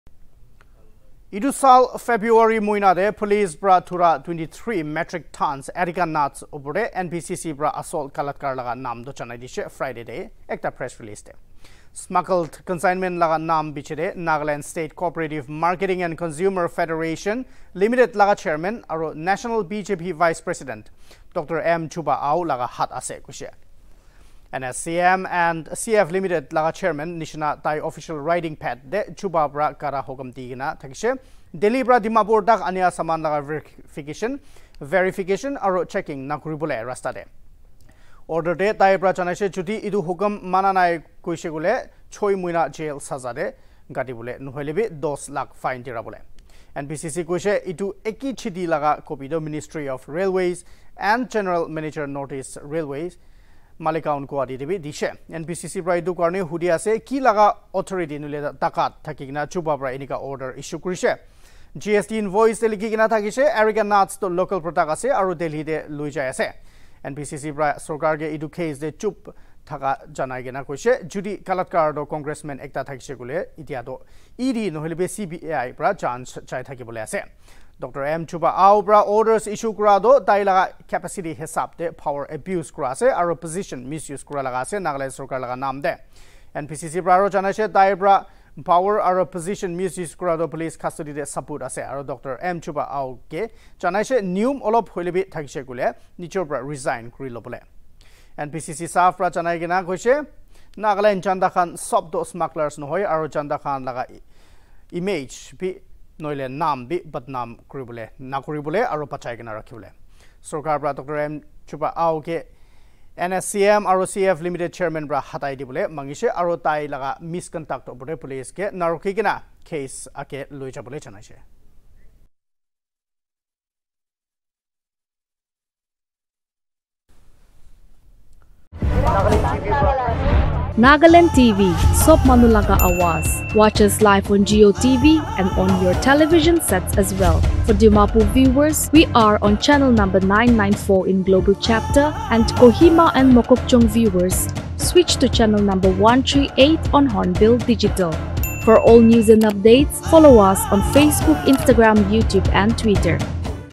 NPCC ISSUES PRESS STATEMENT ON ARECA NUTS ILLEGAL CONSIGNMENT
Nagaland Pradesh Congress Committee (NPCC) on August 26 issued a press statement and has demanded dismissal of BJP National Vice President Dr. M Chuba Ao as chairman of Nagaland State Co-operative Marketing and Consumer Federation (NSCM&CF) Ltd. for abuse of power and misuse of his position